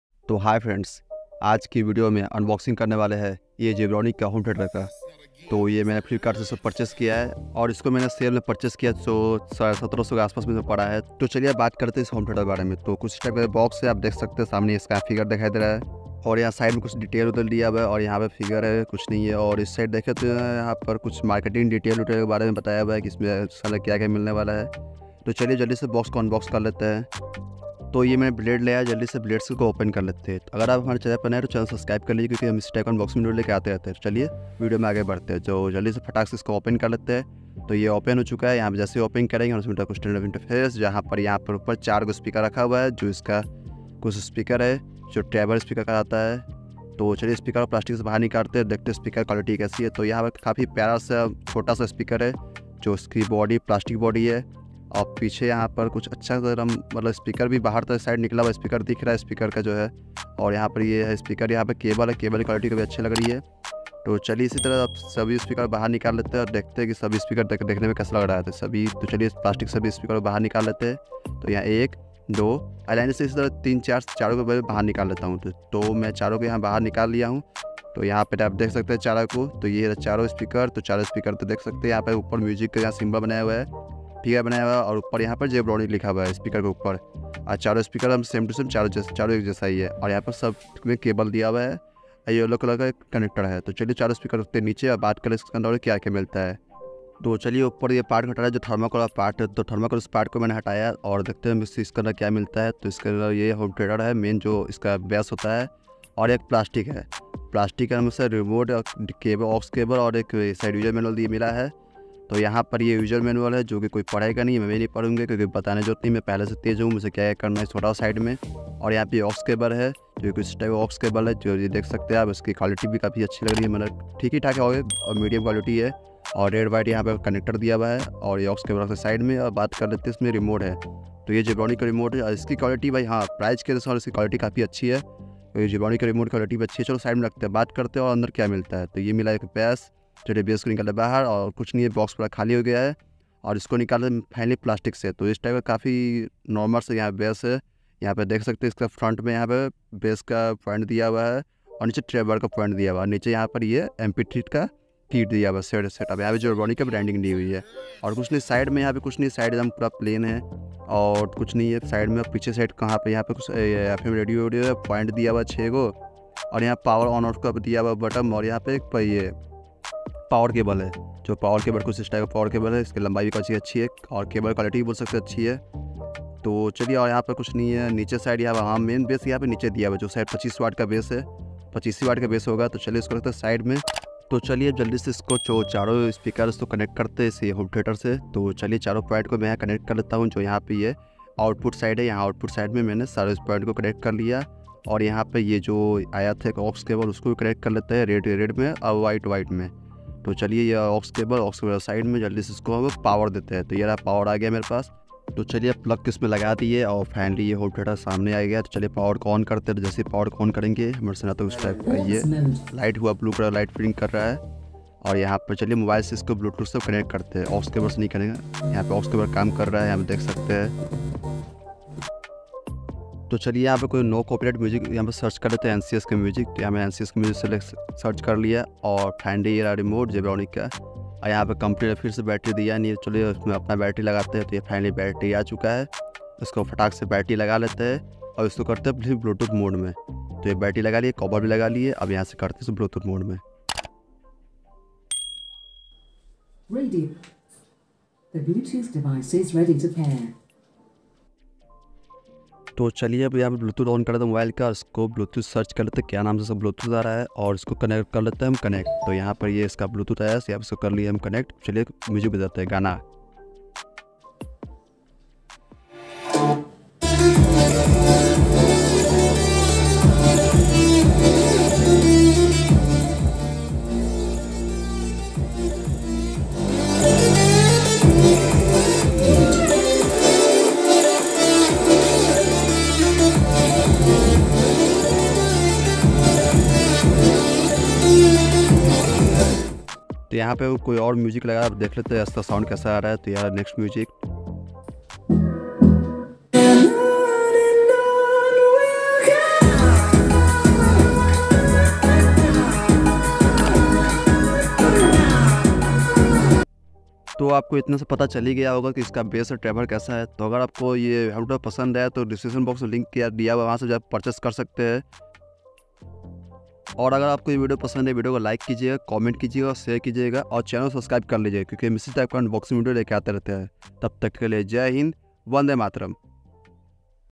Zebronics Zeb Feel 4.1 Speaker Unboxing And Review 🔊|| Zebronics Home Theater
Zebronics Zeb Feel 4 Unboxing And Review 🔊|| Zebronics Home Theater

To iss video me app logo ne dekha ki hamne zebronics zeb feel 4.1 multimedia speaker ka unboxing and review kiya hai. Agar appko bhi zebronics ka home theatre lena hai to app is video ko dekh kar help le sakta hai...

Thanks you soo much. !!!